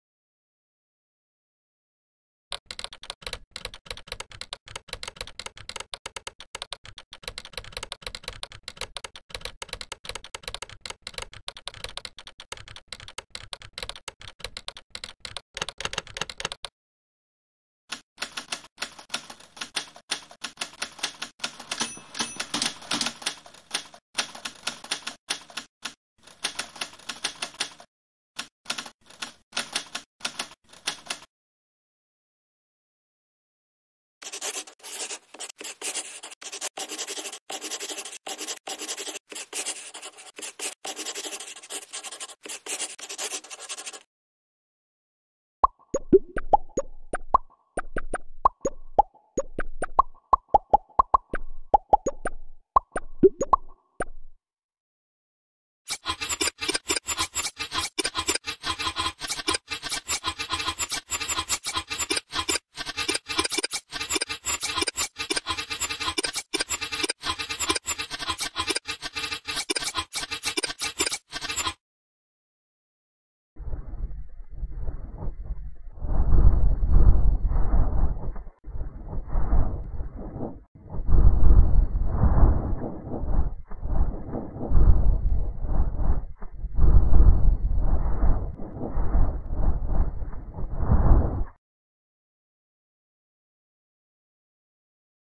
Unity3D - Text Typing Tool (Trailer)
Text Typing Tool is an excellent solution for typing Unity. It is very easy to use. Add the effect can be in a single click, by simply adding a script to the desired object. Text Typing Tool extremely easy to configure and add new effects.

Features:
- Very easy to use UI
- Flexible adjustment effects
- Animation of each character
- Customizable typing cursor
- 6 presets for quick start
- Random sounds for each character